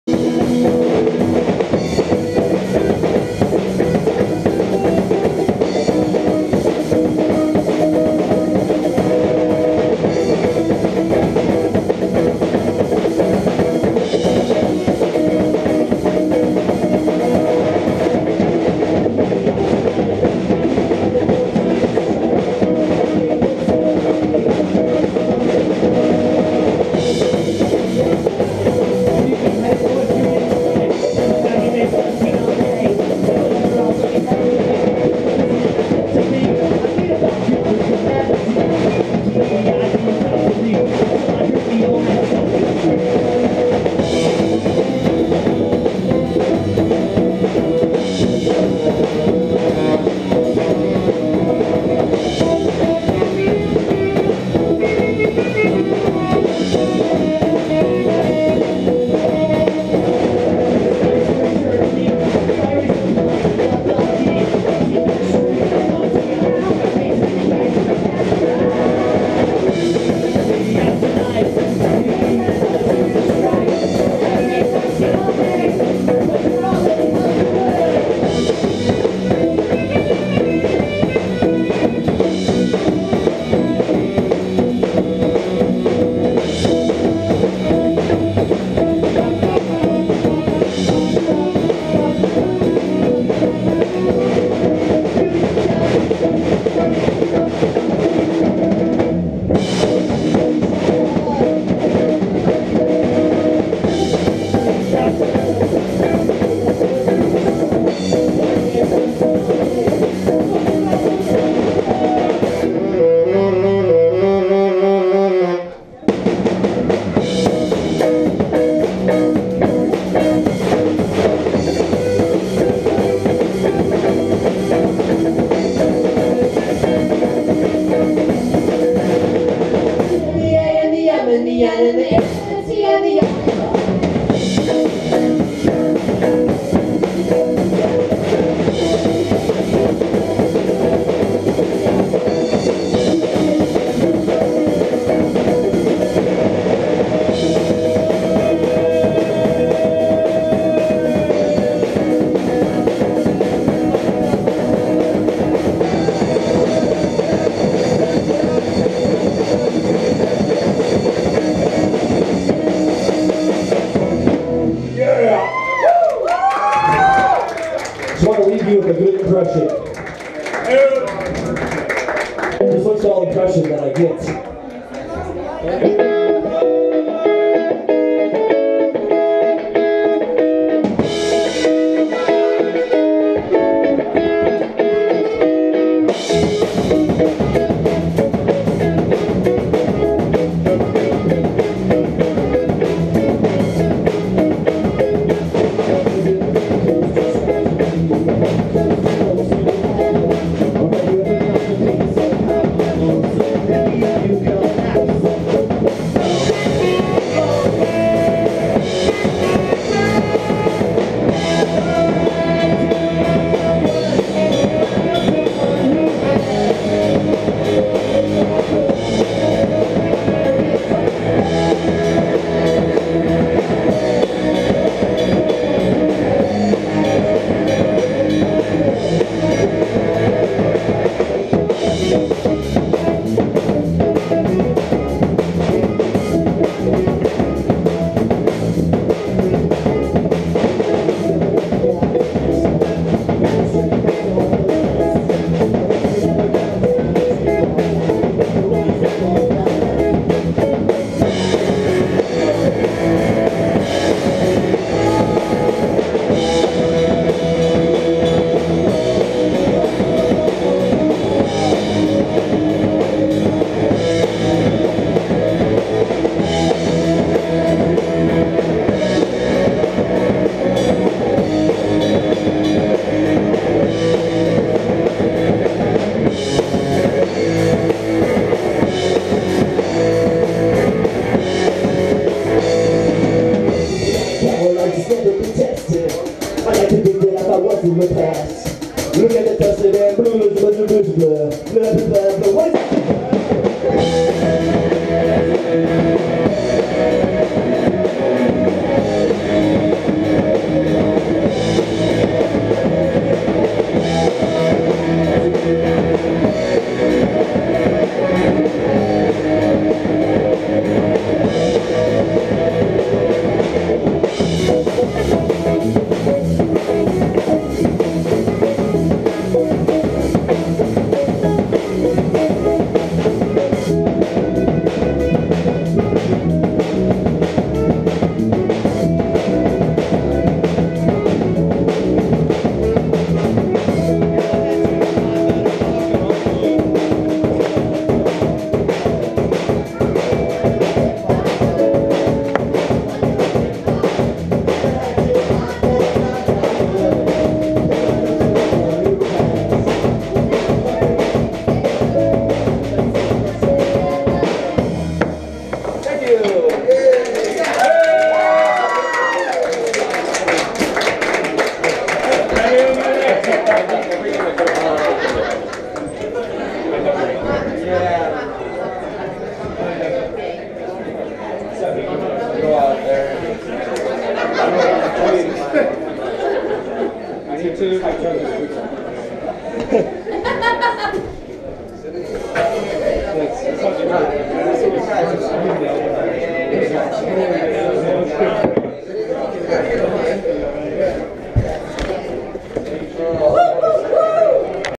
Joystick Generation - Party Rock Cover Band - Buffalo, NY (Part 4 of 4)
Joystick Generation - Party Rock Cover Band - Buffalo, NY LIVE May 7th 2016 at Stamps the Bar Tonawanda, NY